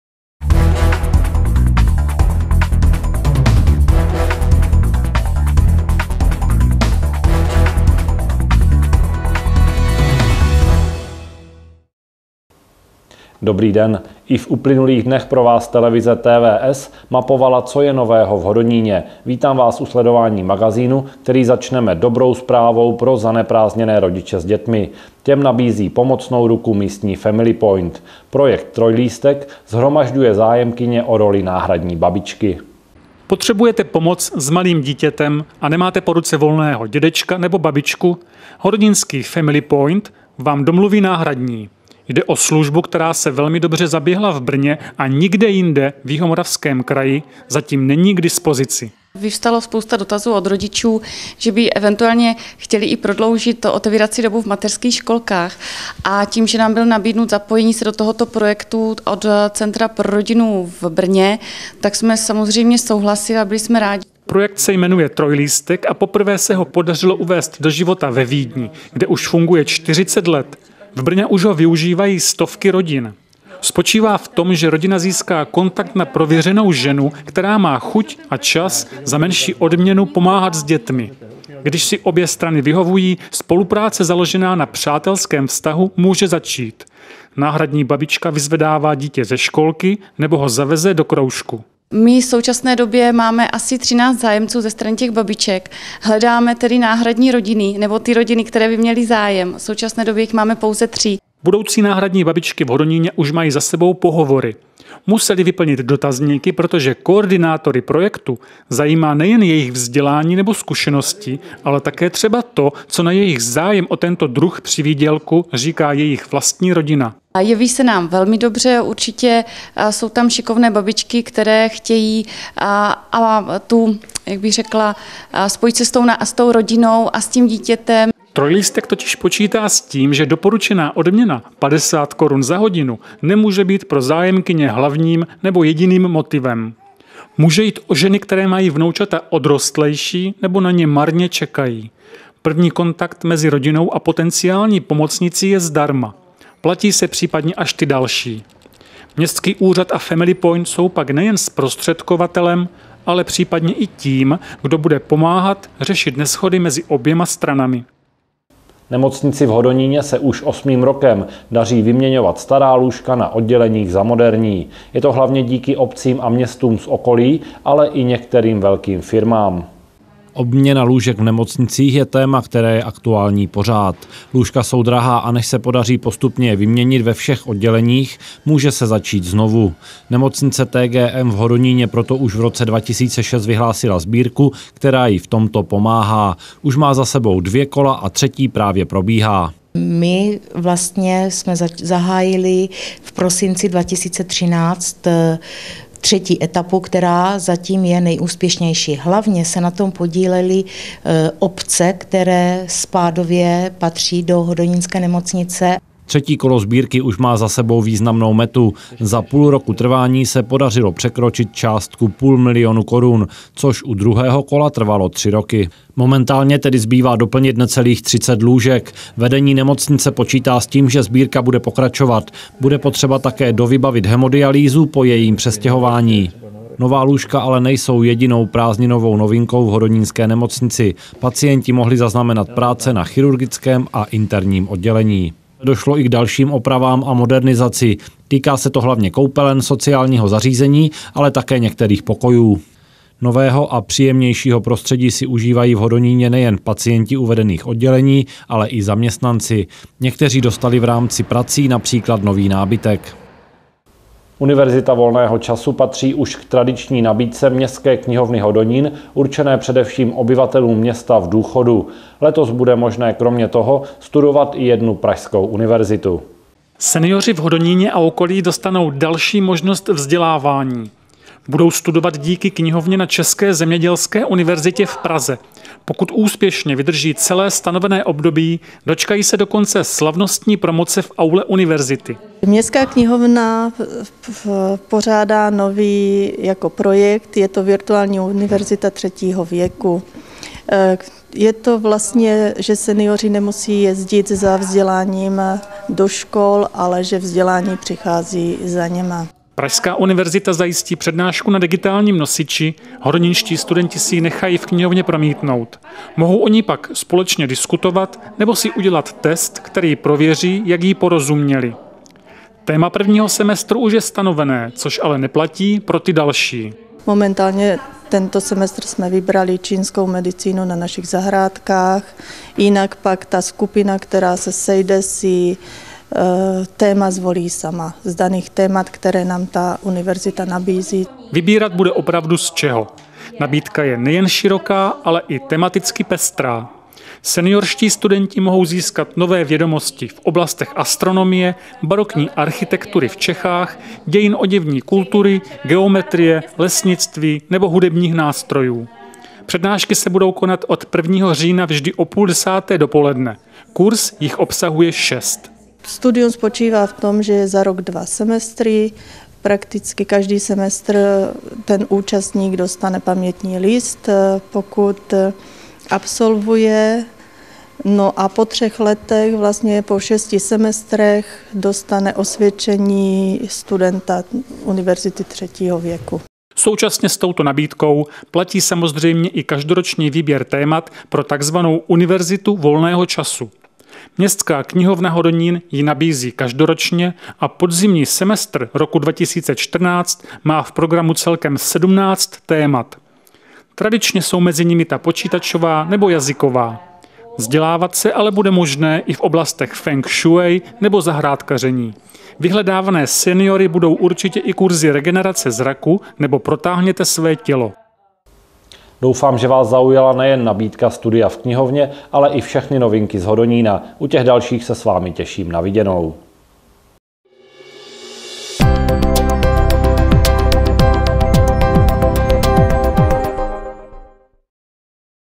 TVS Hodonín - 29. 8. 2014
Hodonín 29. 8.
Family Point hledá rodiny pro náhradní babičky/ Díky obcím se daří měnit lůžka v nemocnici/ Knihovna umožní studovat pražskou univerzitu